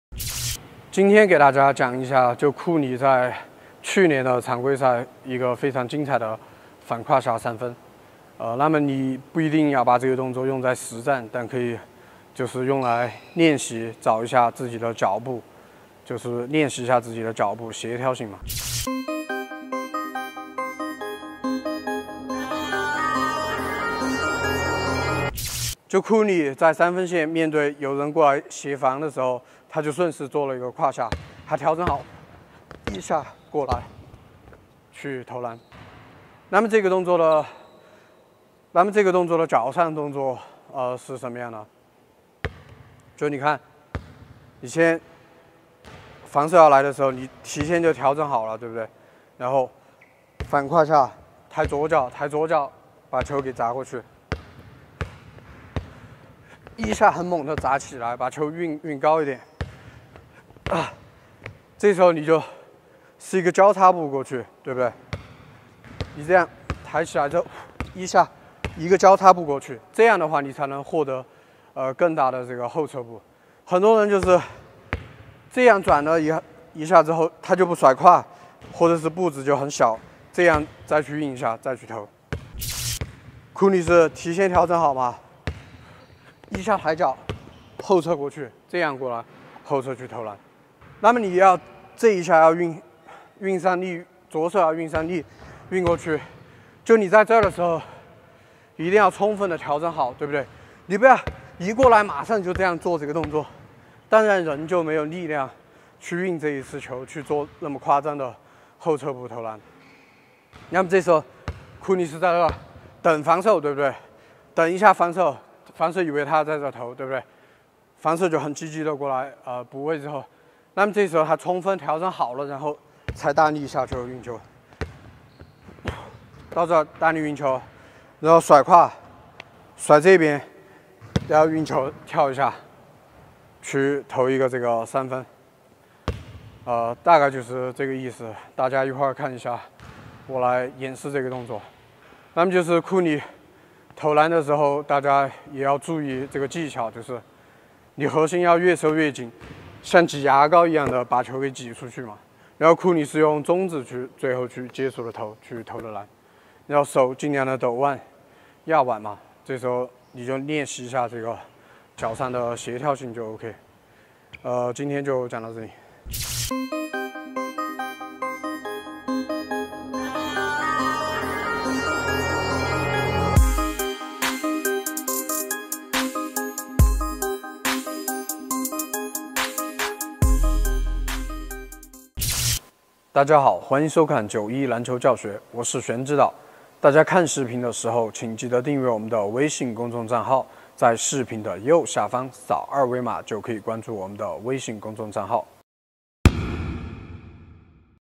91篮球教学 127 库里高难度三分解析
华语篮球教学视频